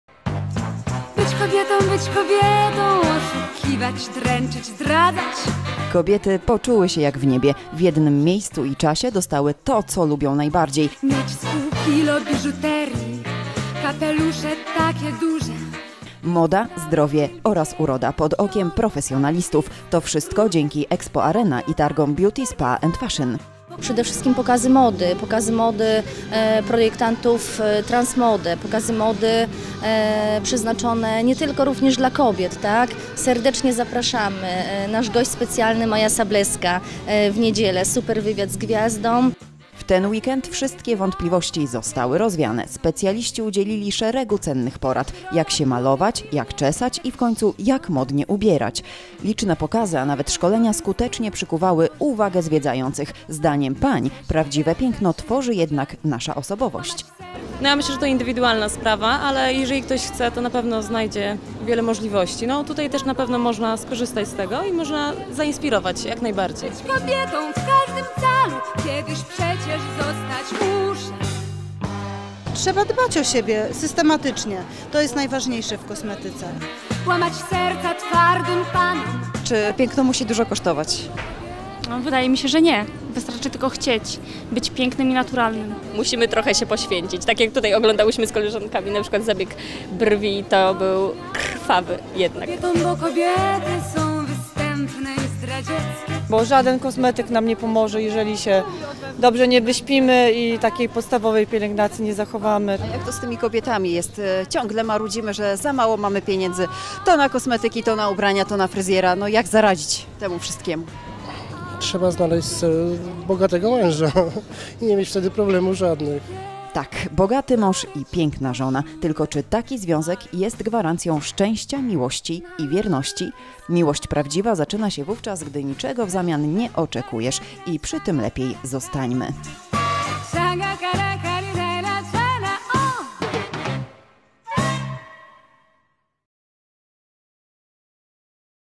BYĆ KOBIETĄ
W jednym miejscu i czasie kobiety dostały to co lubią najbardziej. Moda, zdrowie oraz uroda pod okiem profesjonalistów. To wszystko dzięki Expo Arena i targom Beauty Spa & Fashion.